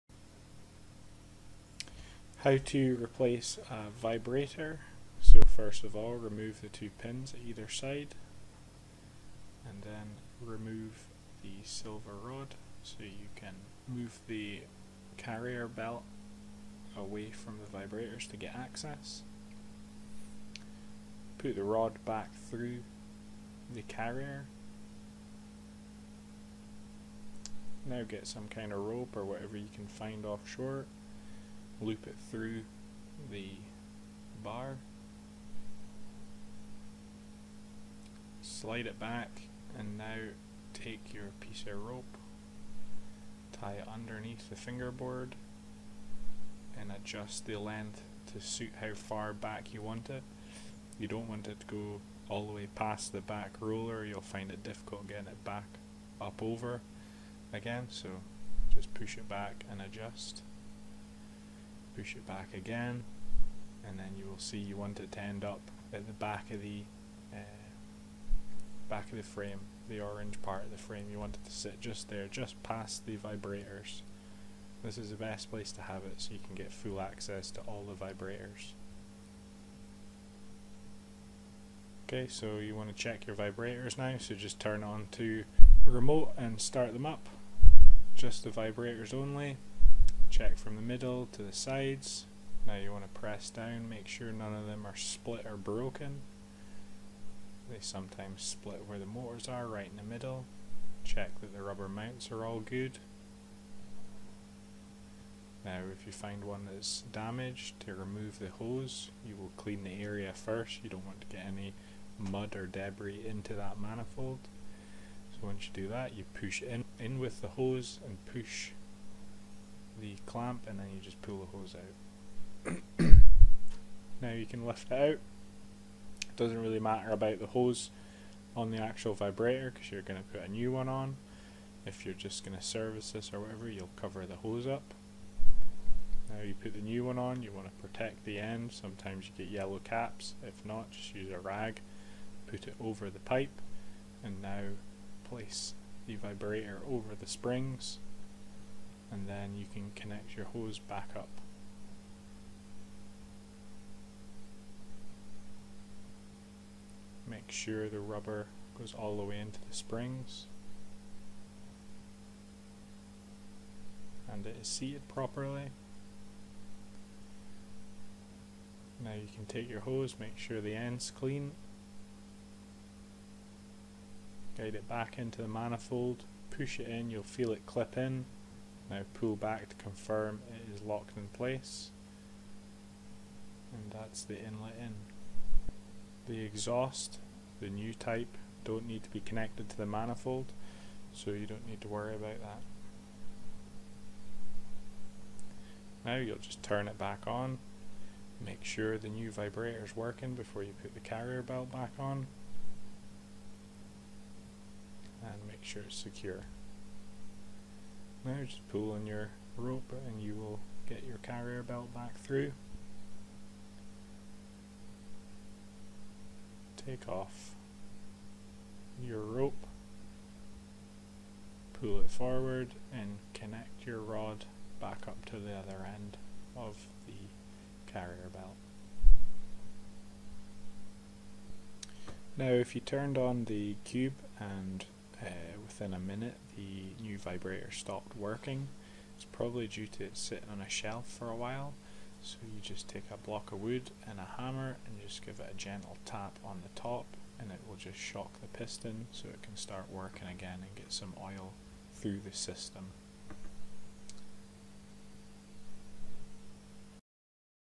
Replacing a micro vibrator on the MudCube
Instructions on how to replace a micro vibrator on the MudCube